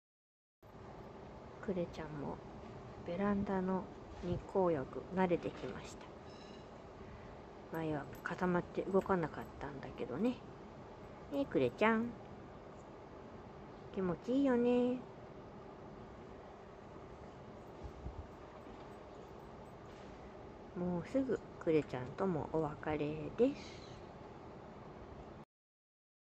懐かしい❗️リハビリ中の旧Clairちゃん
愛猫チャッピーさんの所に来た時は、後ろ脚の筋肉未発達で、JUMPが出来ないけど❗️
私とチャッピーさん二人三脚(四脚)で、
後ろ脚筋肉のリハビリしました❗️
最初ベランダに出た時は、固まり動けず🤣
チャッピーさん全面協力の元徐々に筋肉が
付きました❗️😉😺
それでも慎重派のClairちゃんは、高い所に登る時
必ず私を見てから上ったり
私が、1人で降りれるなら良いよ‼️って伝えると
上がりるの諦めたり。
時々、チャッピーさんに叱られて断念したり
するようになりましたけど
チャッピーさんとの相性もあり里親様へ
譲渡して、今では幸せ🍀な猫生を過ごして
います😉😻